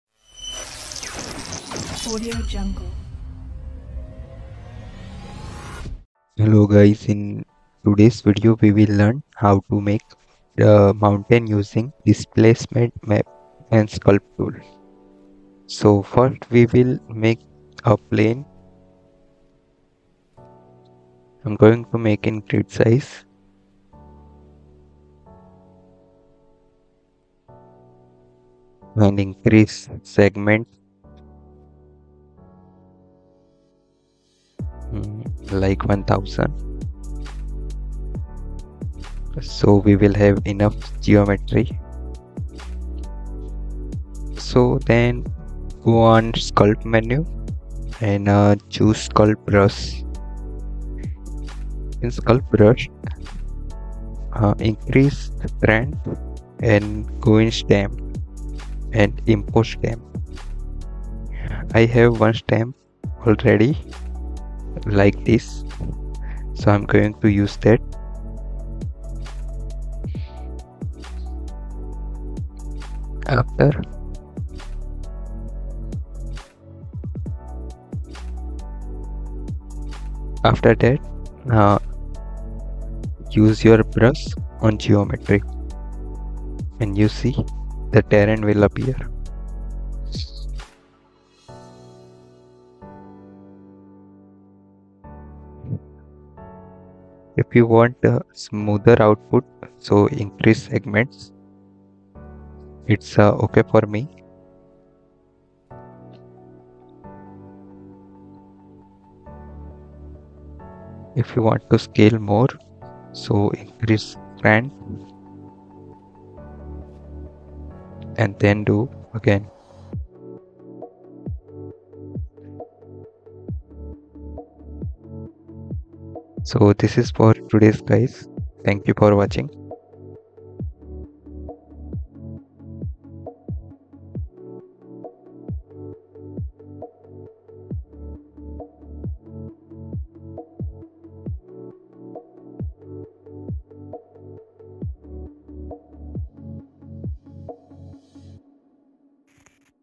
How To Make Mountain in Maya 2023 using Displacement Map and Sculpt Tool | Modeling Tutorial
Here we create game assets and animations  using various software like Autodesk Maya, Mudbox, Zbrush, Substance Painter, Substance Designer, Unity, Unreal Engine etc.

In this video we will learn how make mountain terrain using displacement map and sculpt tool in maya 2023

Create Mountain Terrain in Maya 2023 using Displacement Map and Sculpt Tool | Modeling Tutorial